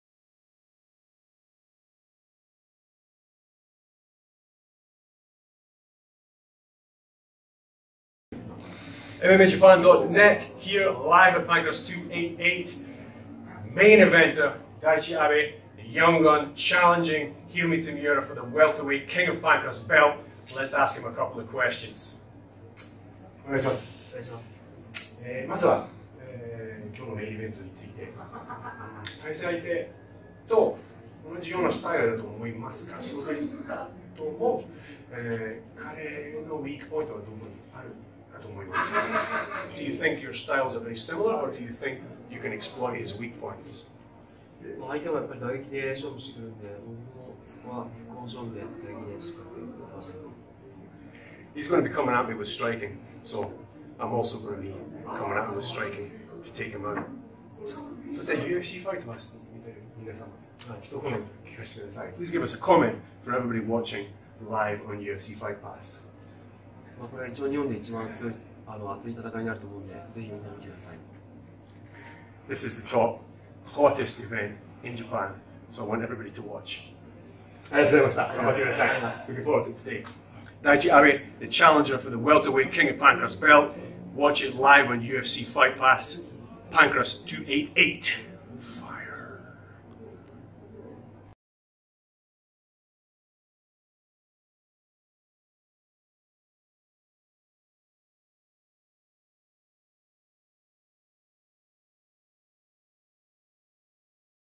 Pancrase 288: Interview with Daiichi Abe
The challenger for the Welter weight King of Pancrase Belt is Daichi Abe. He is on a 3 fight winning streak and is going to be in the Main Event for Pancrase 288.